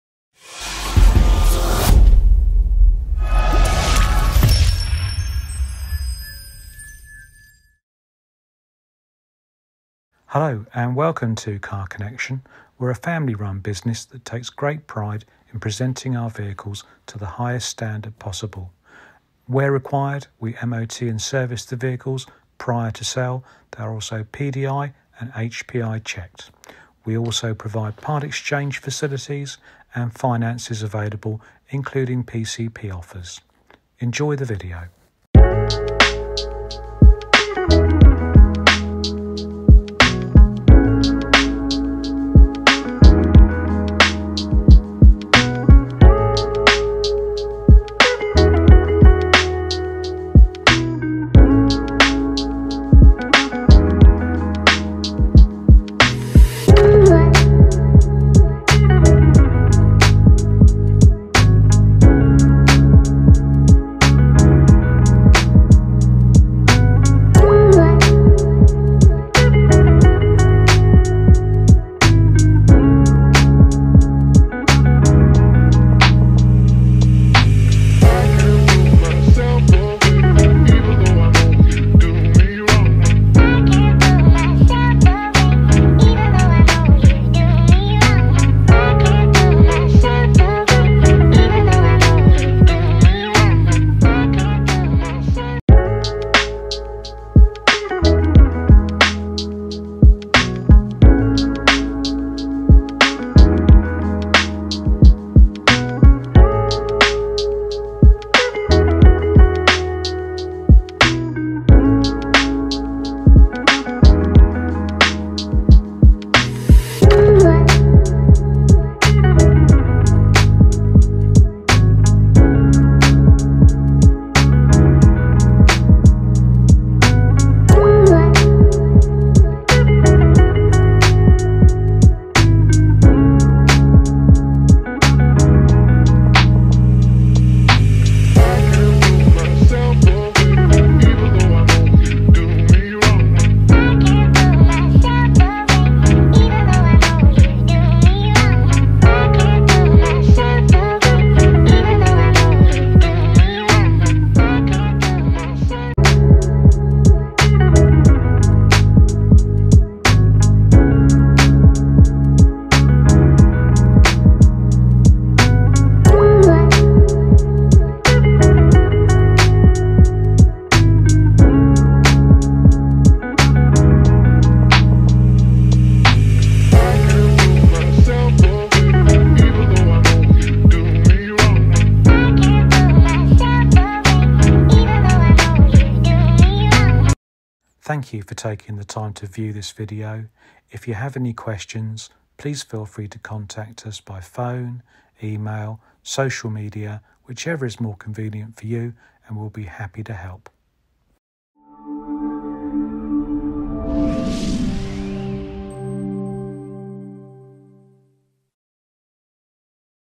2014 CITROEN C4 PICASSO
2014 CITROEN C4 PICASSO 1.6 E-HDI AIRDREAM VTR PLUS ETG6 5d 113 BHP

✅2014
✅ 42,000 miles
✅Diesel
✅1.60 L
✅Automatic

Supplied with MOT and serviced, Warranty Solutions protection included which uniquely includes wear and tear cover.

Ask about our financing plans where this car can be yours from £125.24 p/m